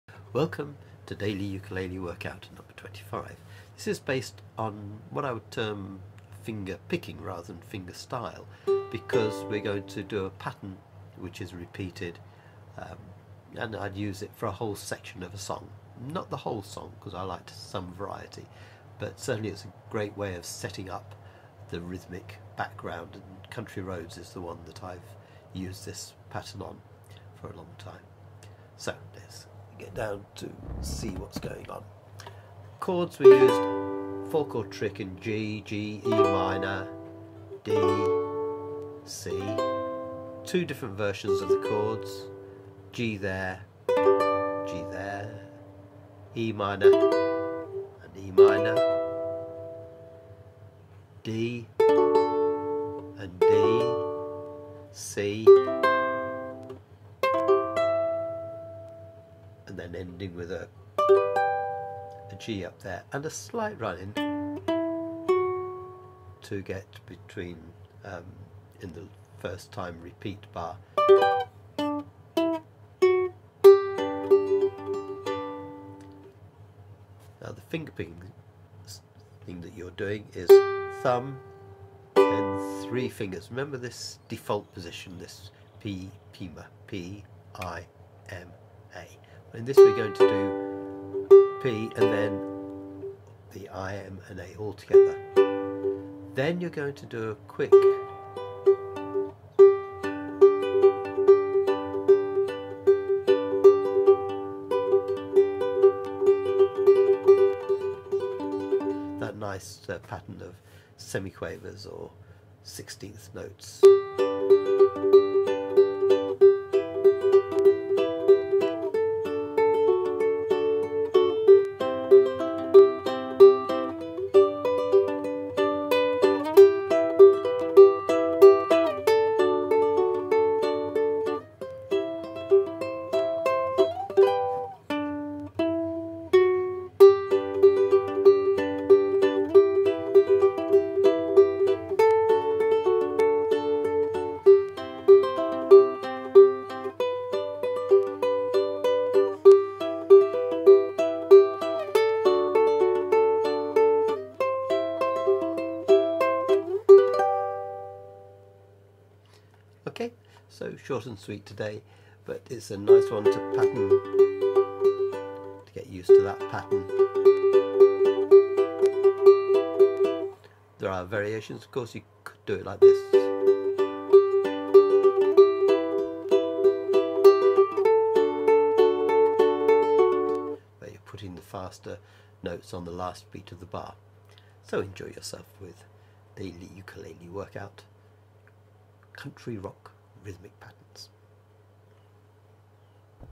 Daily Ukulele Workout 25 - Country rock rhythmic pattern
Delving into finger picking in this workout